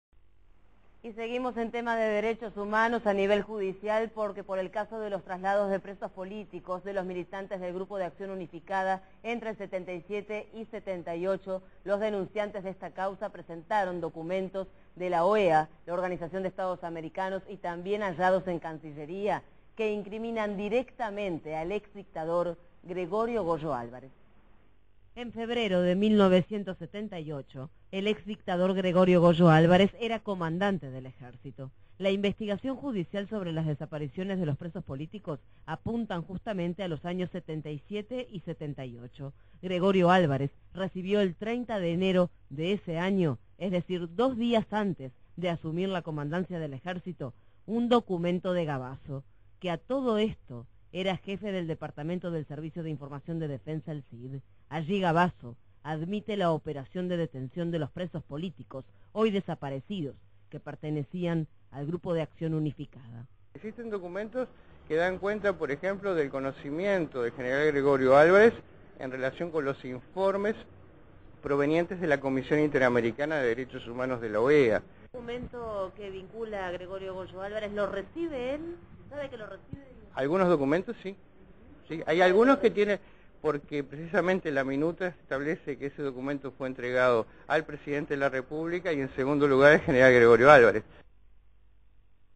EL GOYO ARRINCONADO
DOCUMENTOS DE OEA Y CANCILLERÍA PRUEBAN LA PARTICIPACIÓN DEL GOYO ALVAREZ EN LA REPRESION